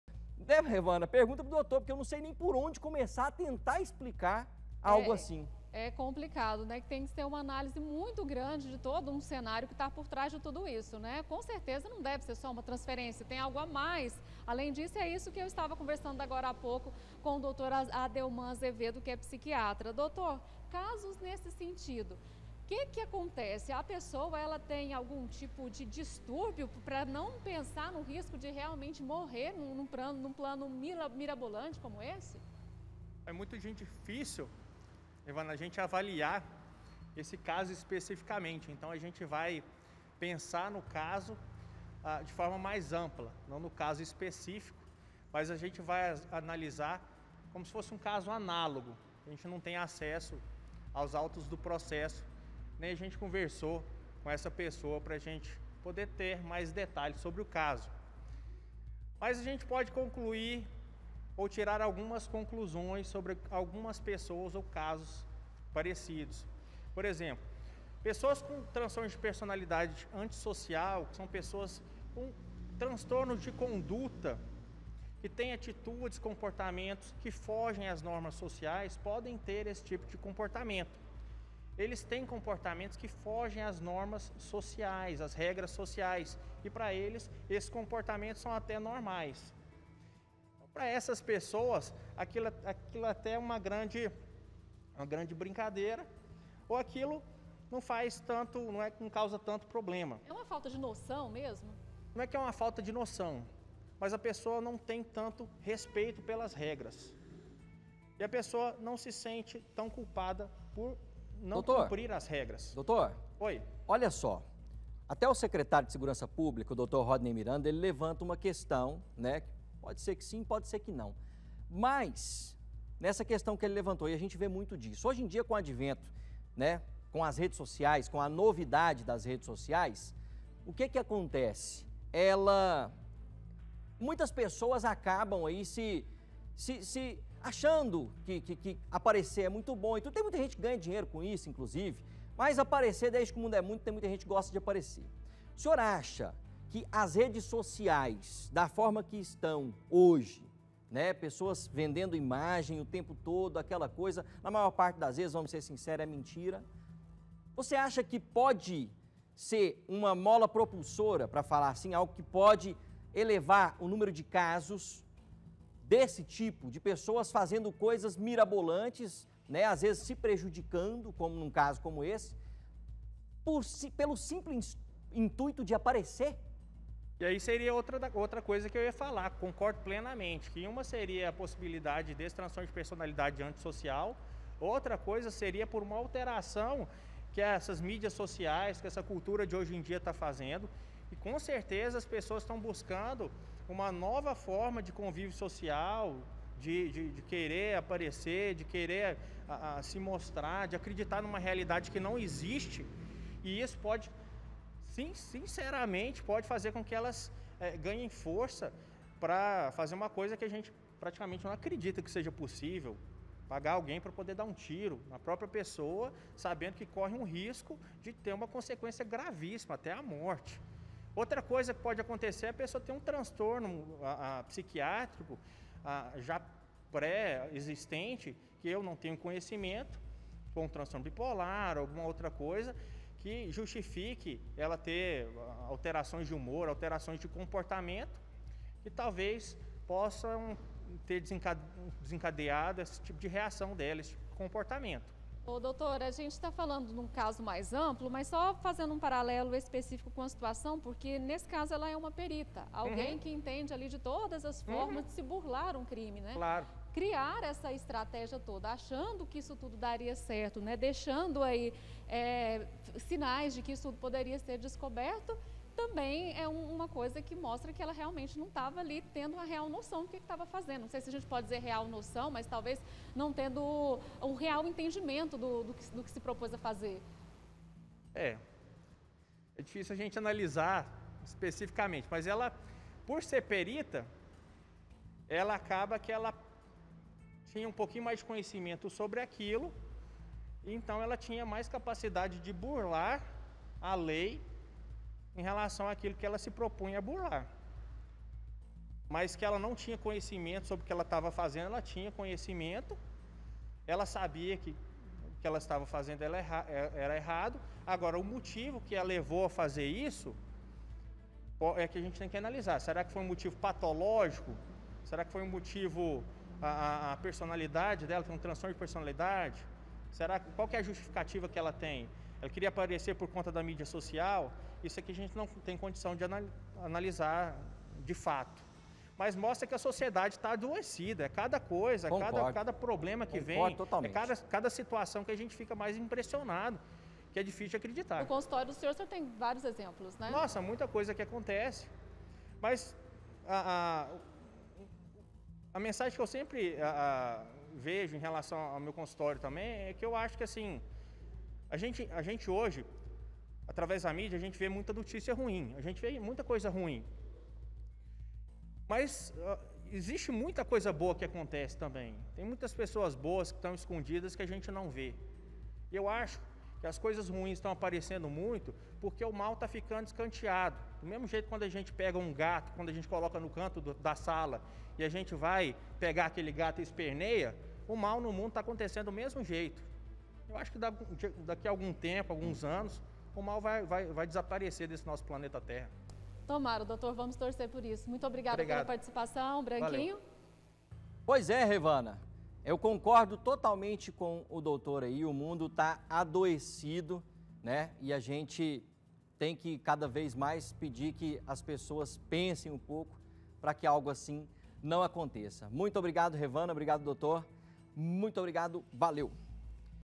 TENTATIVA DE HOMICÍDIO: O QUE LEVA UMA PESSOA A FORJAR UM CRIME?
A REVANA OLIVEIRA ESTÁ COM UM PSIQUIATRA PRA GENTE TENTAR ENTENDER O QUE LEVA UMA PESSOA A FORJAR UMA TENTATIVA DE HOMICÍDIO.